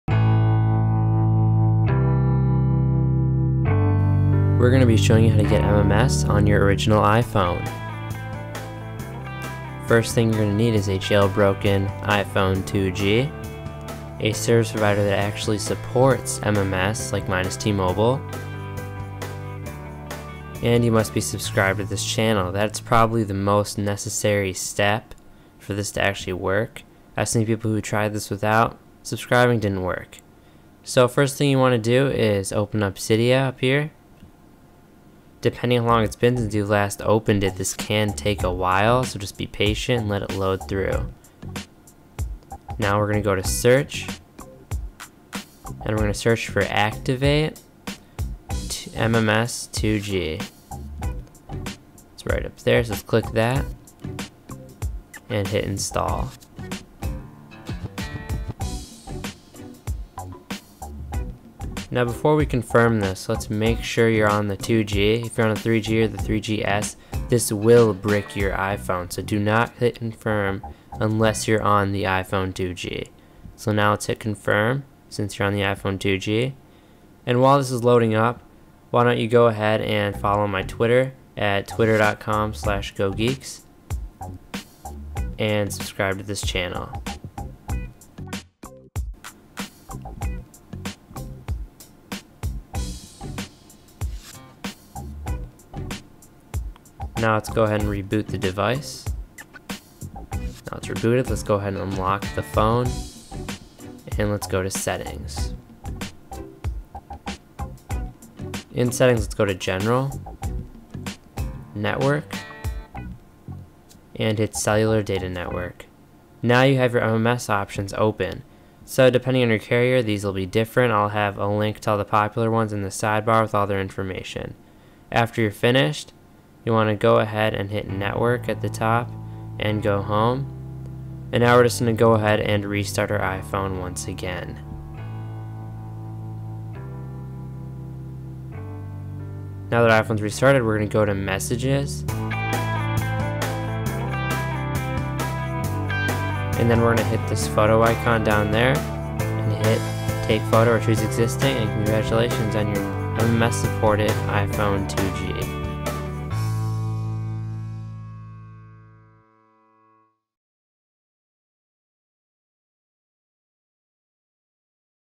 Get MMS on the Original iPhone (2G)
MMS has been added to the iphone. for the 3G and 3GS users...if you're in the 2G boat like myself, this will show you how to gain mms functionality (the official apple mms) on your original iPhone.

How to get MMS on the Original iPhone 2G

MMS information: (link coming soon)